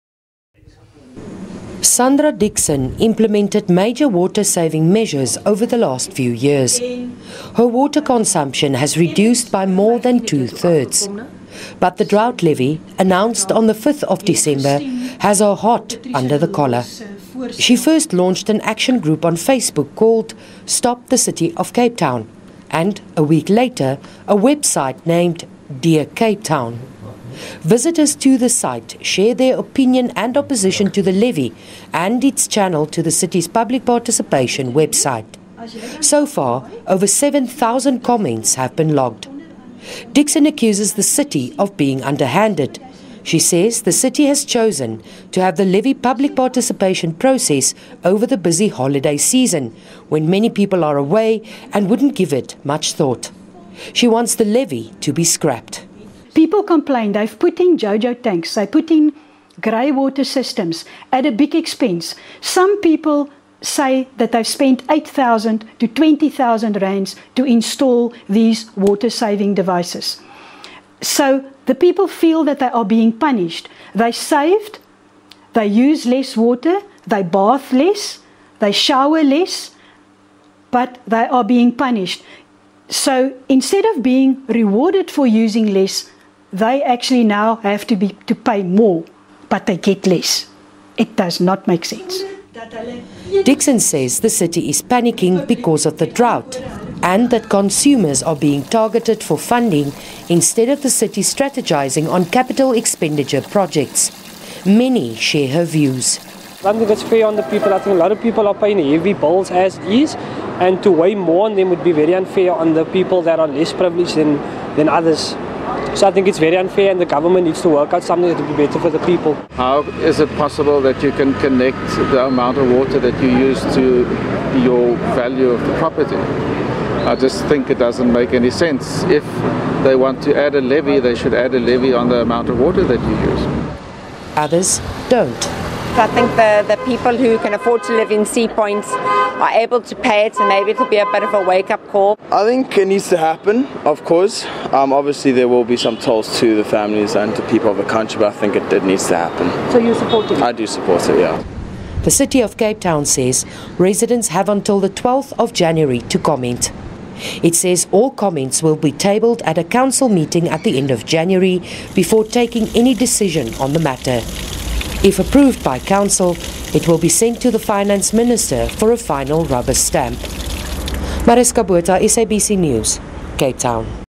Brackenfell resident up in arms over drought levy proposal
There is growing opposition to the City of Cape Town's proposed drought levy. The city aims to levy property owners for water usage according to the value of their properties. The levy will pay for essential projects to make new water available. A resident of Brackenfell has formed an action group to take on the City.